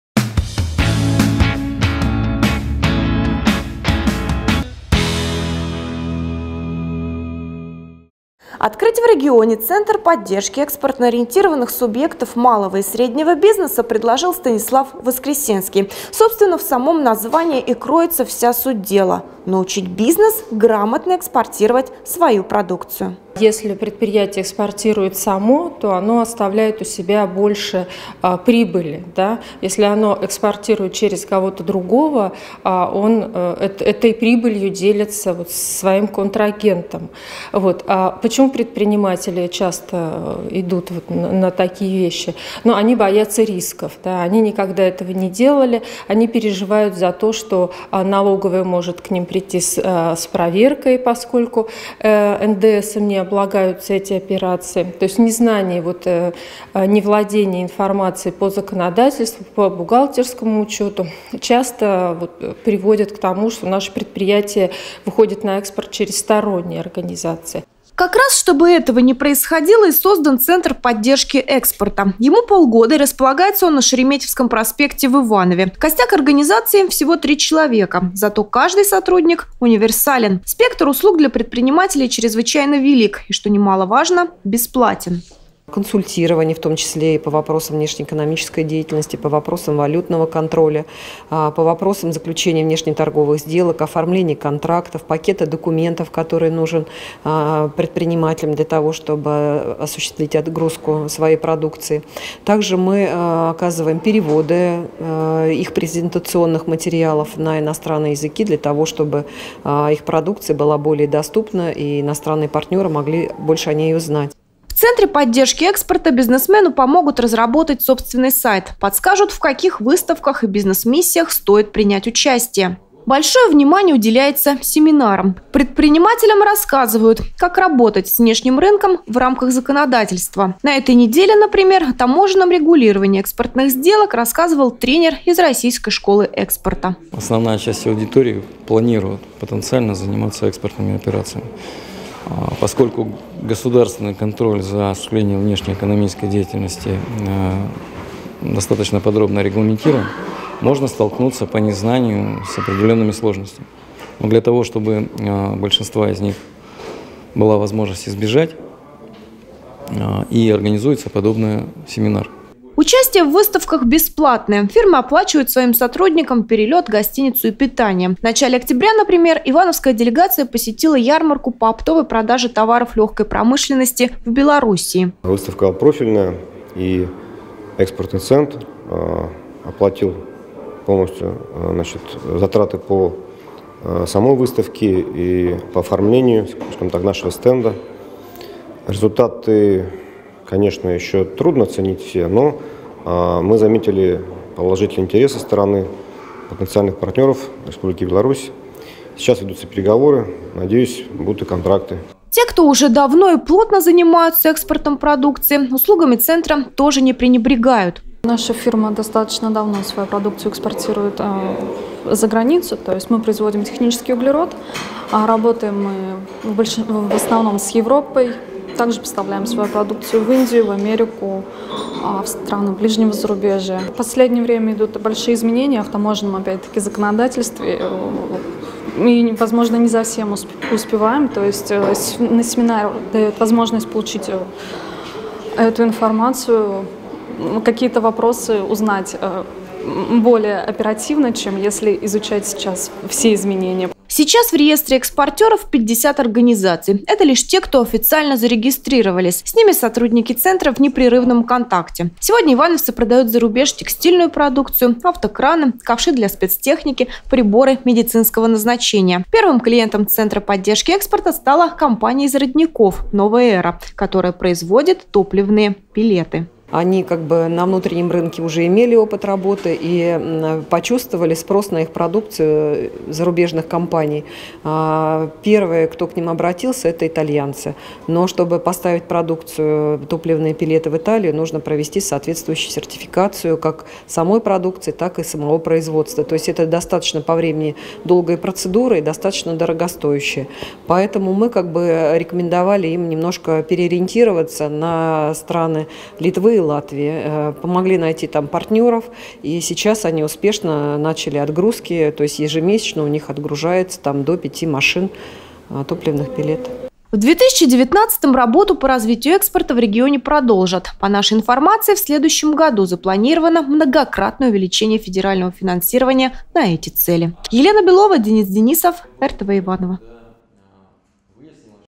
Чему научат в центре поддержки экспорта?
Открыть в регионе центр поддержки экспортно-ориентированных субъектов малого и среднего бизнеса предложил Станислав Воскресенский. И вот уже полгода организация успешно выполняет свою задачу. Какую пользу она приносит предпринимателям – смотрите в нашем специальном репортаже.